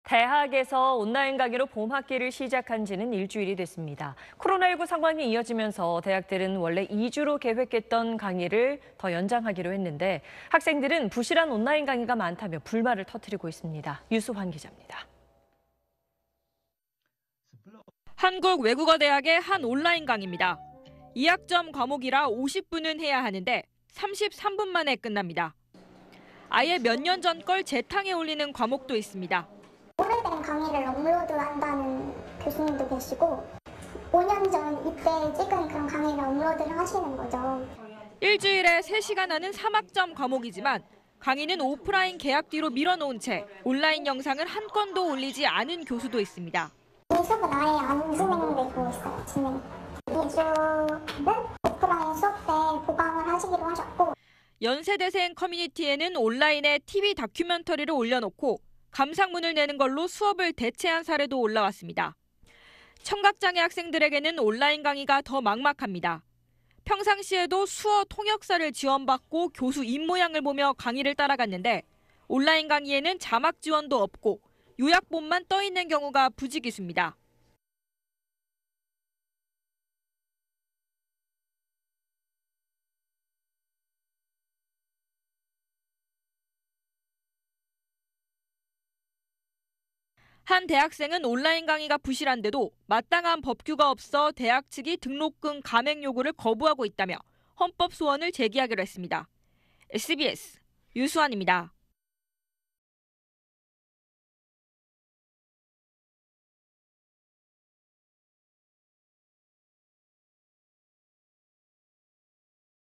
몇 년 전 강의 재탕까지…부실한 인강에 대학생들 분통 / SBS
대학에서 온라인 강의로 봄학기를 시작한 지는 일주일이 됐습니다. 코로나19 상황이 이어지면서 대학들은 원래 2주로 계획했던 온라인 강의를 더 연장하기로 했는데 학생들은 부실한 온라인 강의가 많다며 불만을 터트리고 있습니다.
유수환 기자입니다.